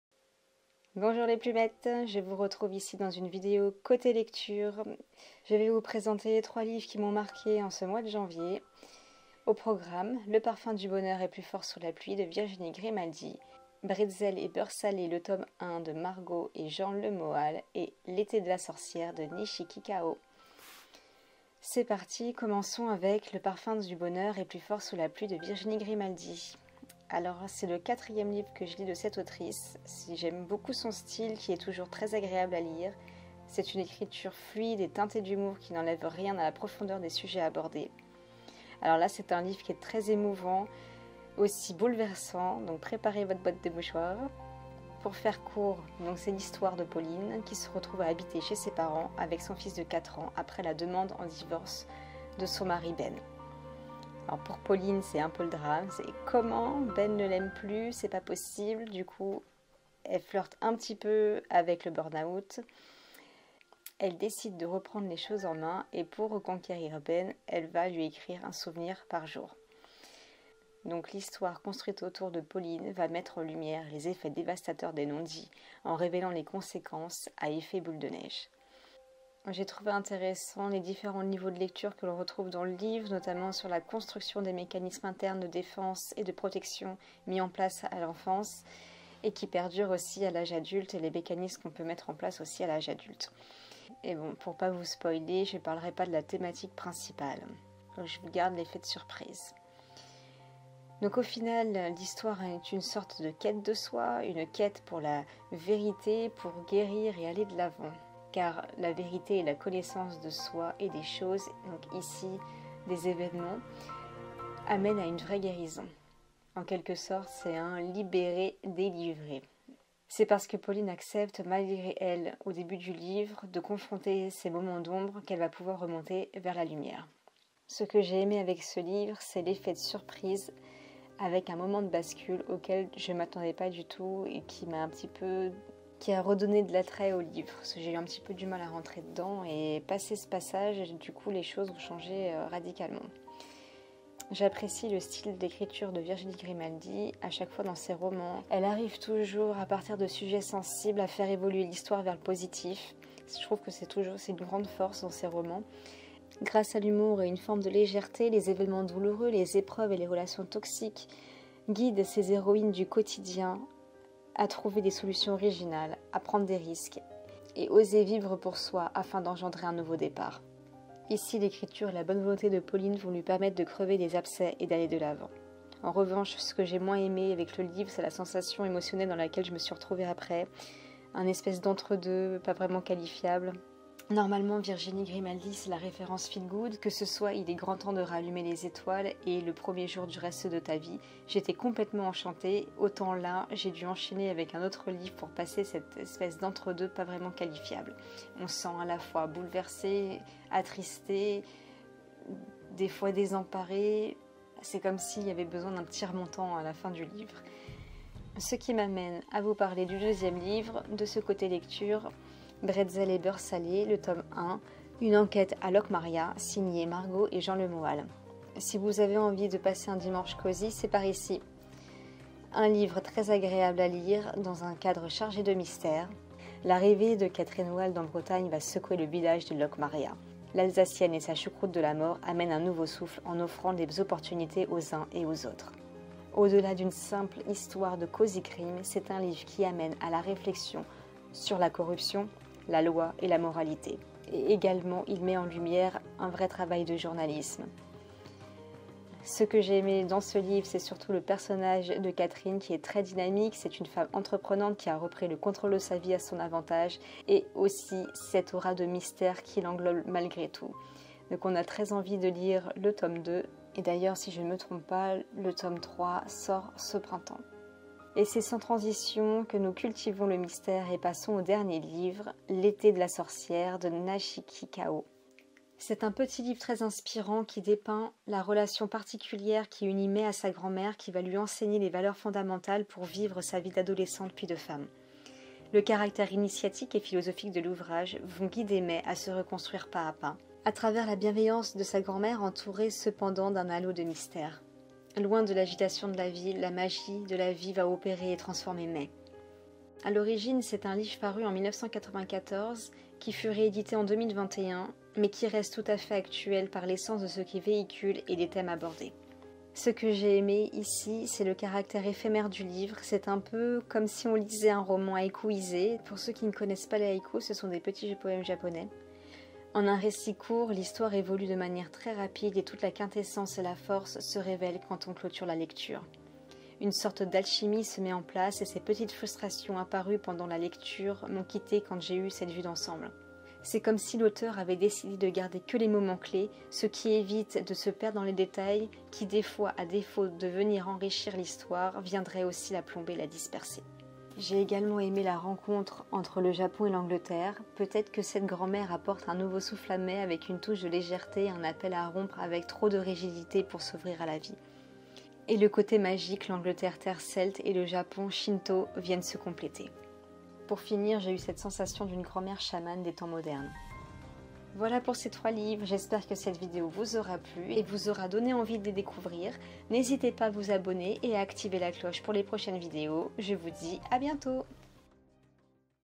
Côté Lecture - janvier 2022
Dans cette vidéo je vous présente 3 lectures du mois de janvier, c'est parti !

Chapitre :
00:00 Intro
00:21 ☔️ Le parfum du bonheur est plus fort sous la pluie - Virginie Grimaldi
03:28  🥨 Bretzel et Beurre Salé, T1 - Margot & Jean Le Moal
04:32 🌞 L' Été de la sorcière - NASHIKI Kaho